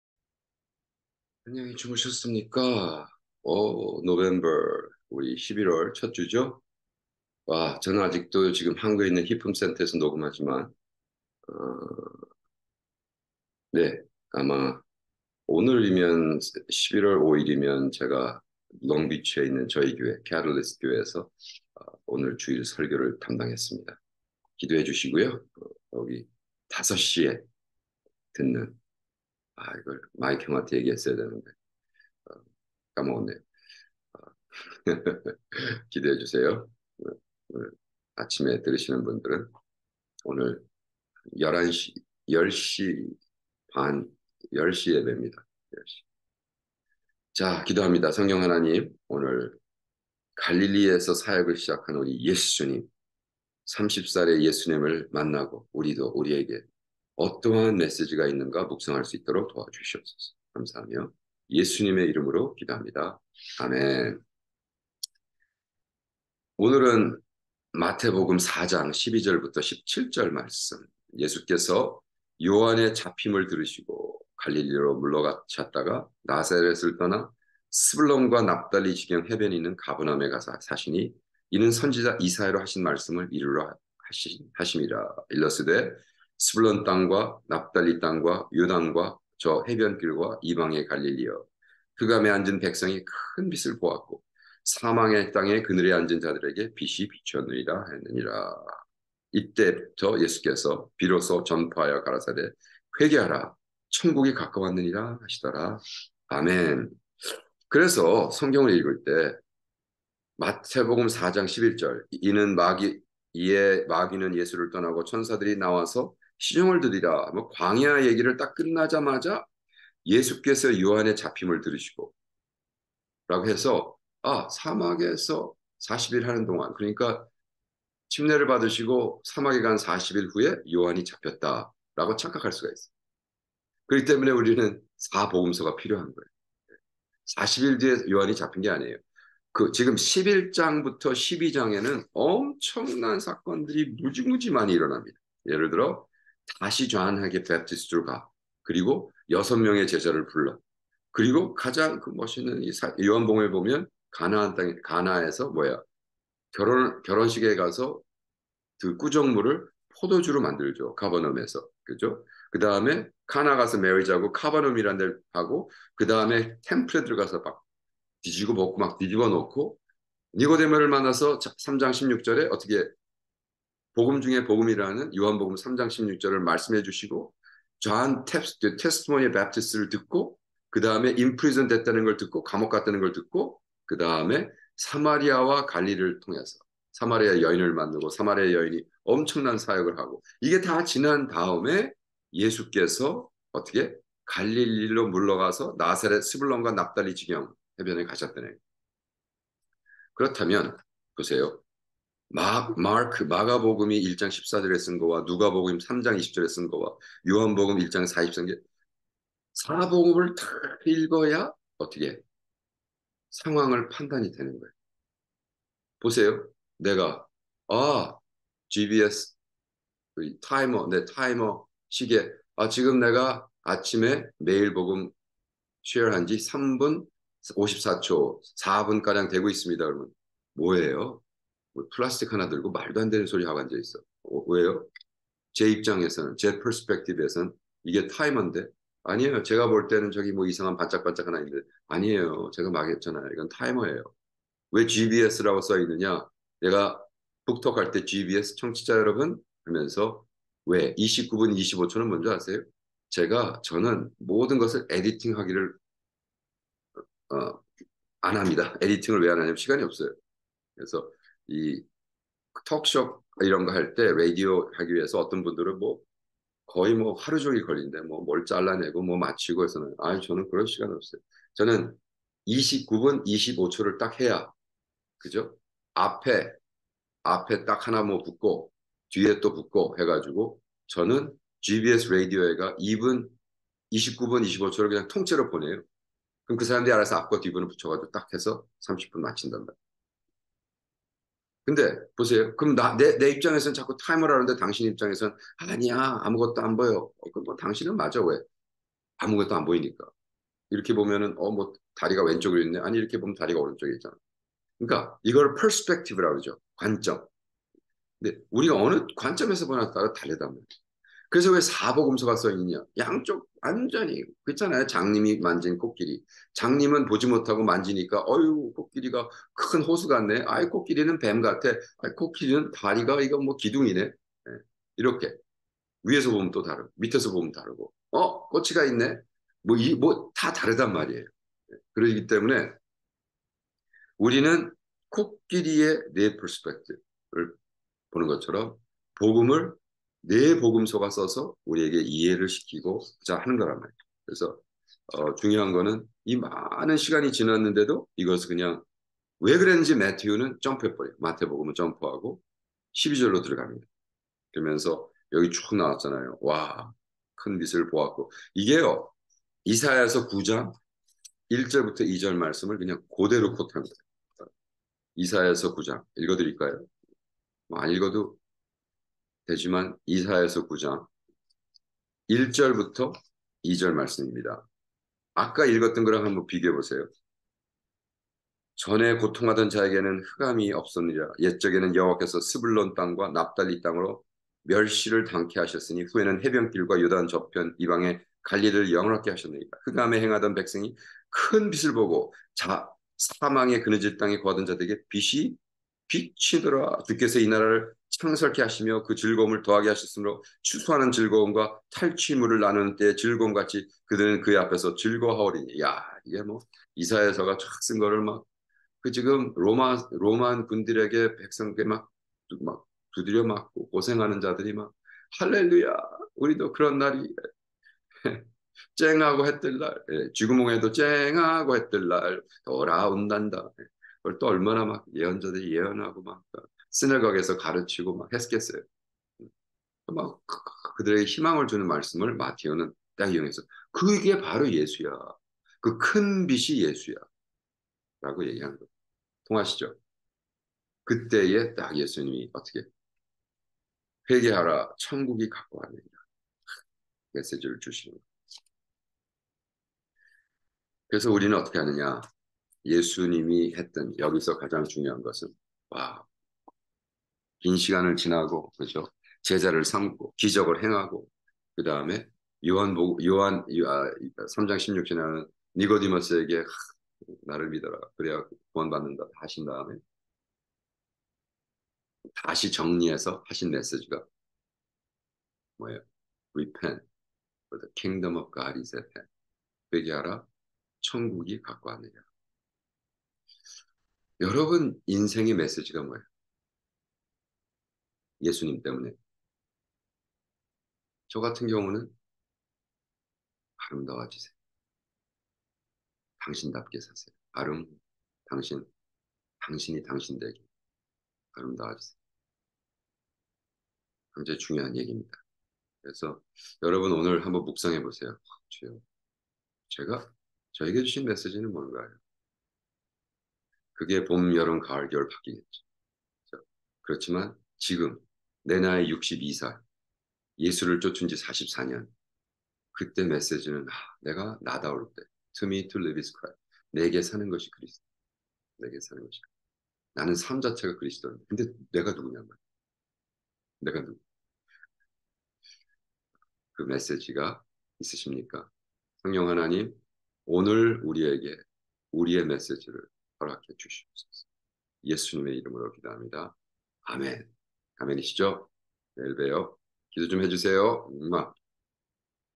매일 복음 1139일: 예수님이 사역을 시작하시다! 마태 4:12-17. 2023년 11월 5일 (주일)
닥터 밥오 TV 방문을 환영 합니다.